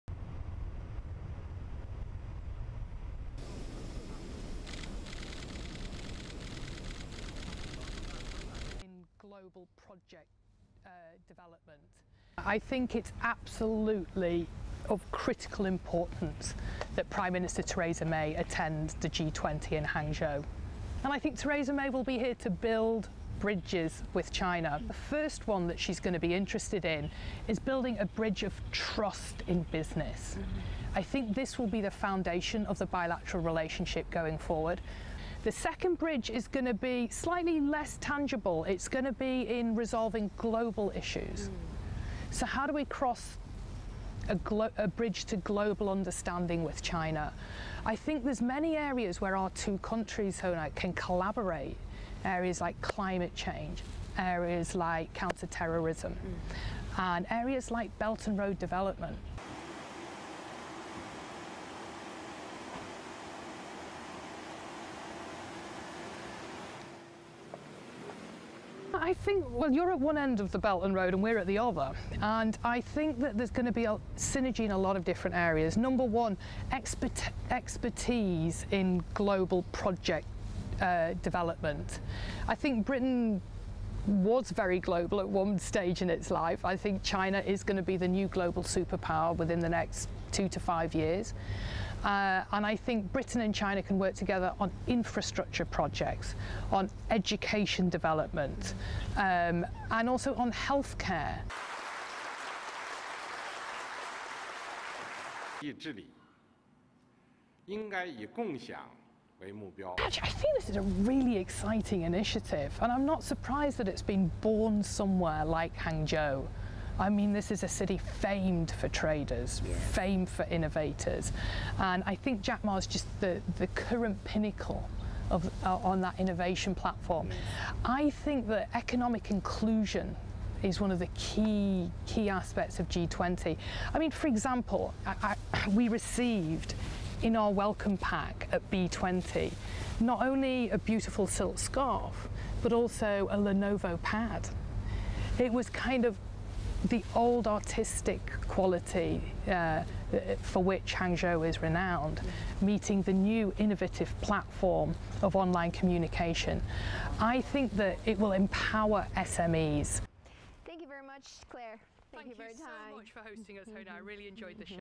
British PM's Attendance at G20 Summit Expected to Boost Bilateral Ties with China
British Prime Minister Teresa May's attendance at the Group of Twenty (G20) Summit in China will boost China-UK bilateral ties, British Chamber of Commerce (BCC) Chair Clare Pearson told China Central Television (CCTV) recently.